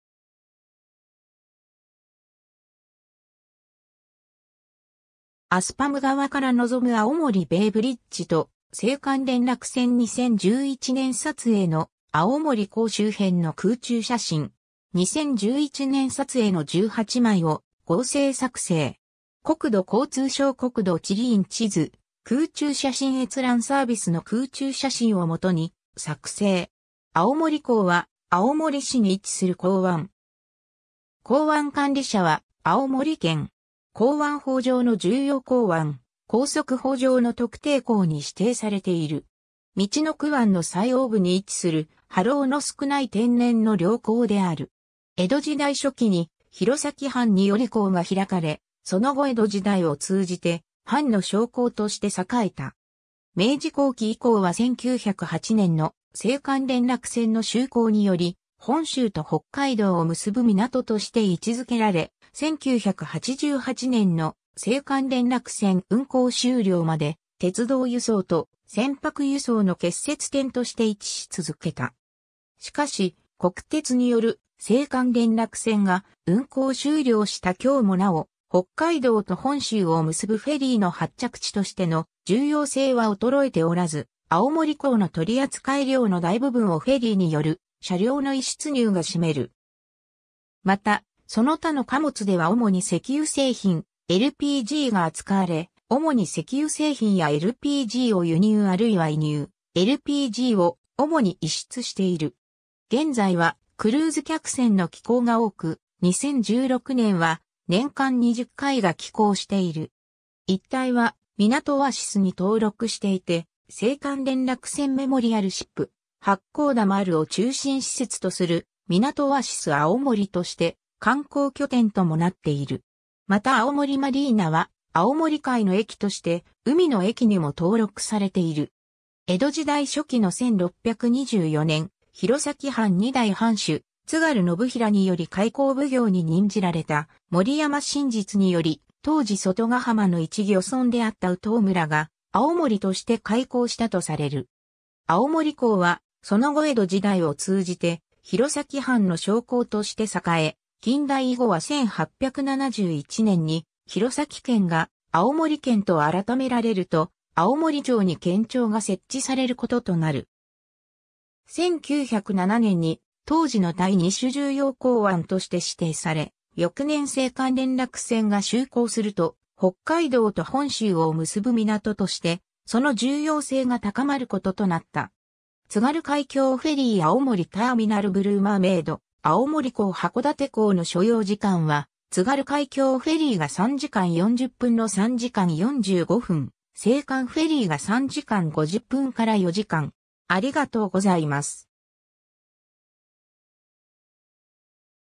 青森港
アスパム側から望む青森ベイブリッジと青函連絡船 2011年撮影の青森港周辺の空中写真。
2011年撮影の18枚を合成作成。
 青森港（あおもりこう）は、青森市に位置する港湾。
港湾管理者は青森県。
港湾法上の重要港湾、港則法上の特定港に指定されている。
 陸奥湾の最奥部に位置する、波浪の少ない天然の良港である。
江戸時代初期に弘前藩により港が開かれ、その後江戸時代を通じて藩の商港として栄えた。
明治後期以降は1908年（明治41年）の青函連絡船の就航により本州と北海道を結ぶ港として位置付けられ、1988年（昭和63年）の青函連絡船運航終了まで鉄道輸送と船舶輸送の結節点として位置し続けた。
しかし、国鉄による青函連絡船が運航終了した今日もなお、北海道と本州を結ぶフェリーの発着地としての重要性は衰えておらず、青森港の取扱量の大部分をフェリーによる車両の移出入が占める。
また、その他の貨物では主に石油製品・LPGが扱われ、主に石油製品やLPGを輸入あるいは移入、LPGを主に移出している。
 現在はクルーズ客船の寄港が多く、2016年（平成28年）は年間20回（9隻）が寄港している。
 一帯はみなとオアシスに登録していて、青函連絡船メモリアルシップ八甲田丸を中心施設とするみなとオアシスあおもりとして観光拠点ともなっている。
また青森マリーナはあおもり海の駅として海の駅にも登録されている。
 江戸時代初期の1624年（寛永元年）、弘前藩2代藩主津軽信枚により開港奉行に任じられた森山信実により、当時外ヶ浜の一漁村であった善知鳥（うとう）村が「青森」として開港したとされる。
青森港はその後江戸時代を通じて弘前藩の商港として栄え、近代以後は1871年（明治4年）に弘前県が青森県と改められると、青森町に県庁が設置されることとなる。
1907年（明治40年）に当時の第2種重要港湾として指定され、翌年青函連絡船が就航すると、北海道と本州を結ぶ港としてその重要性が高まることとなった。
 津軽海峡フェリー 青森ターミナル （高速船ターミナル時代） ブルーマーメイド 青森港～函館港の所要時間は、津軽海峡フェリーが3時間40分-3時間45分、青函フェリーが3時間50分-4時間（使用船舶や運航ダイヤによって異なる）。